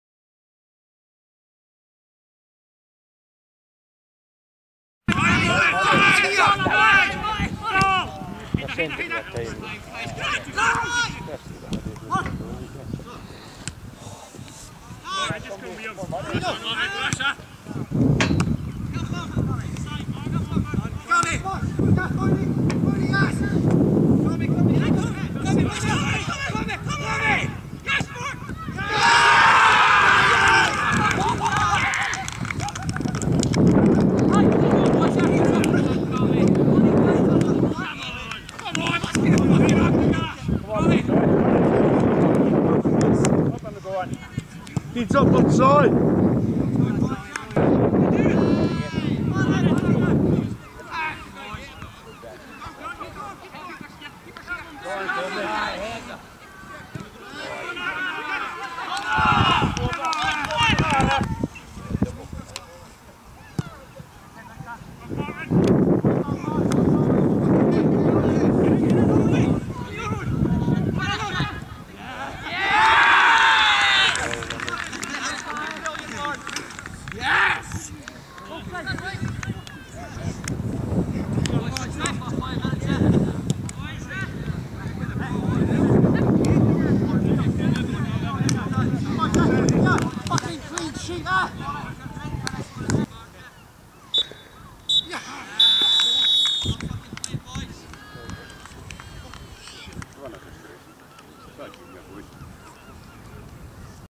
Bishopmill Villa v Portsoy - Goal Highlights
Footage of the goals in this Highland welfare cup semi final tie at Merson Park Buckie between Moray welfare league sides Bishopmill Villa and Portsoy. Full Highlights to follow